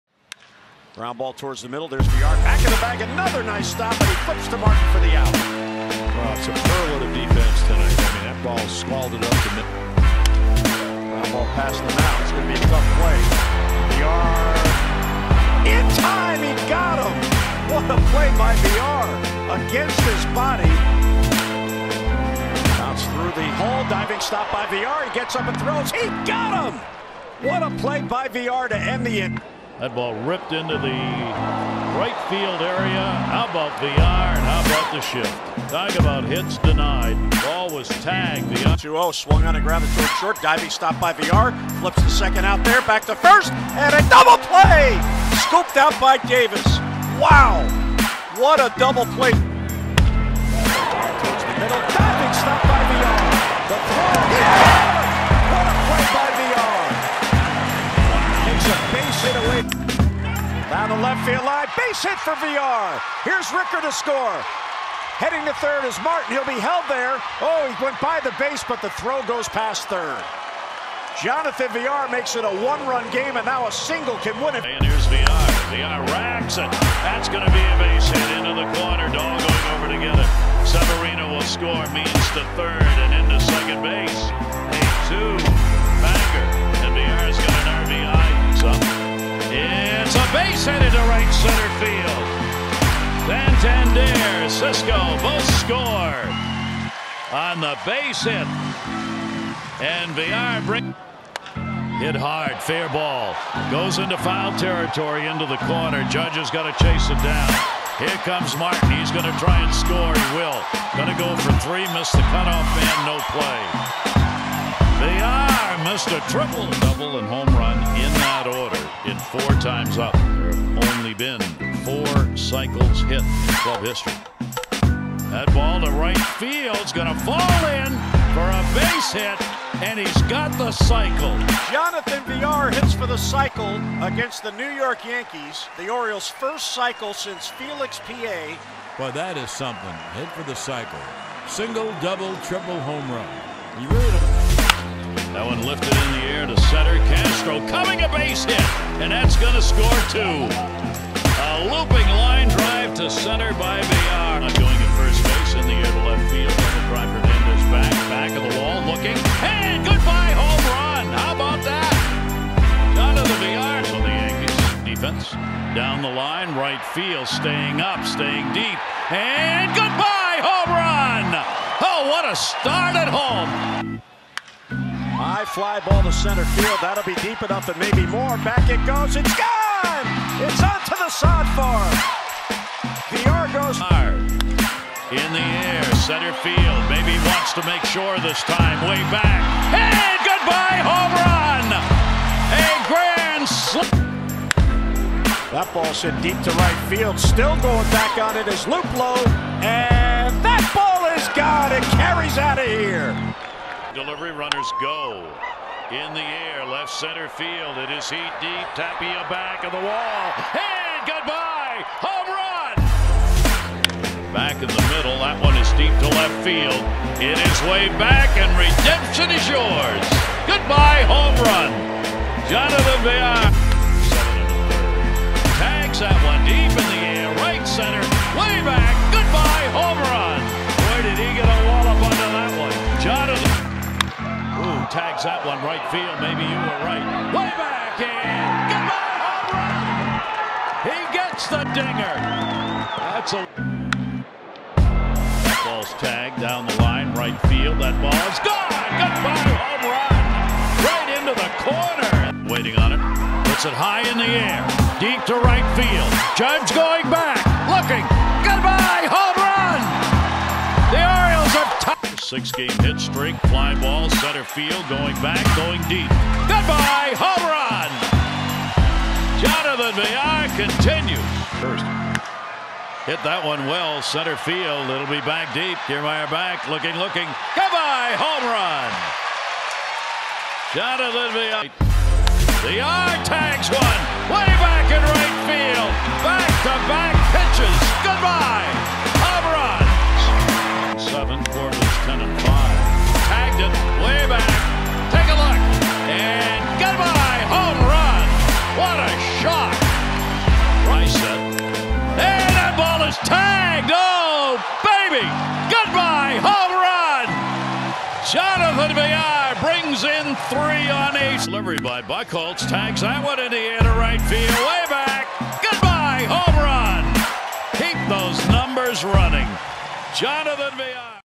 Jonathan Villar 2019 Orioles Highlights
Stats: .274 BA | 24 HR | 73 RBI | .792 OPS